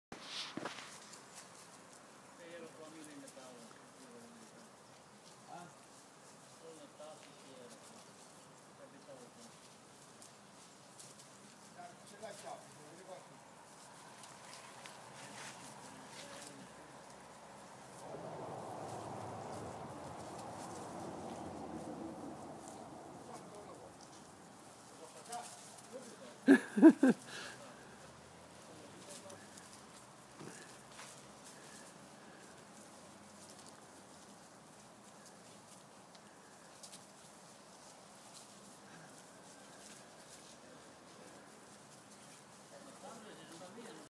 Cinghiali a Trieste 3
Mi prendono in giro: uno fa finta di esser morto...